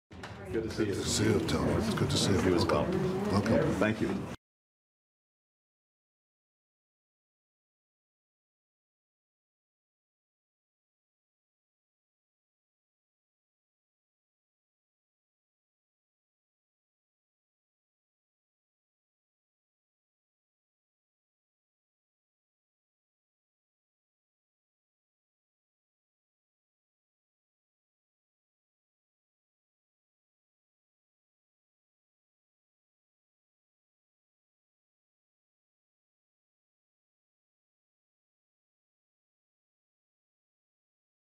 Blinken holds bilateral meeting with Israeli PM Netanyahu
Blinken holds bilateral meeting with Israeli PM Netanyahu:  US Secretary of State Antony Blinken holds a bilateral meeting with Israeli Prime Minister Benjamin Netanyahu in Jerusalem. The head of US diplomacy, Antony Blinken, urged Monday in Jerusalem to implement "urgent measures" to stop the new spiral of violence between Israelis and Palestinians.